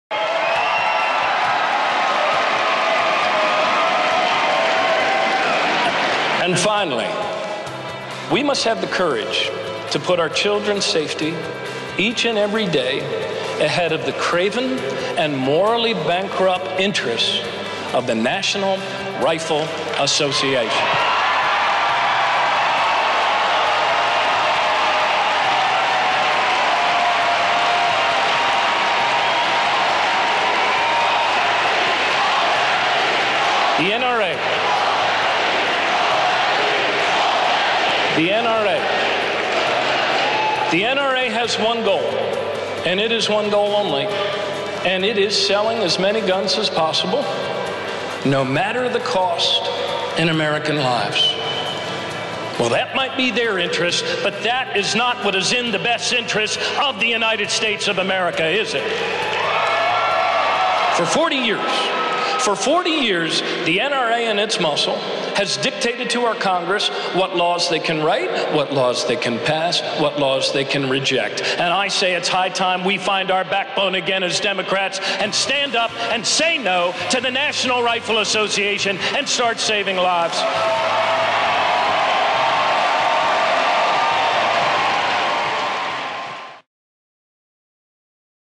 Iowa JJ Dinner: It's time we stand up and say "no" to the NRA
Martin O'Malley speaks at the Iowa Jefferson Jackson Dinner.